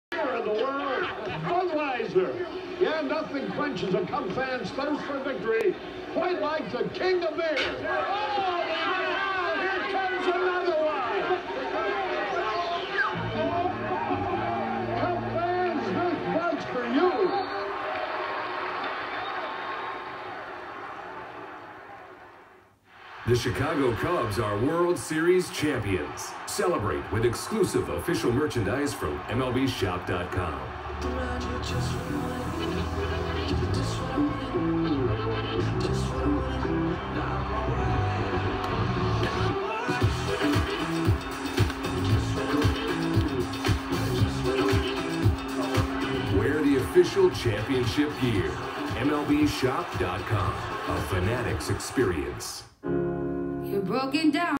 This Buds For You
Commercial airing minutes after Chicago Cubs won 2016 World Series with Harry Carty.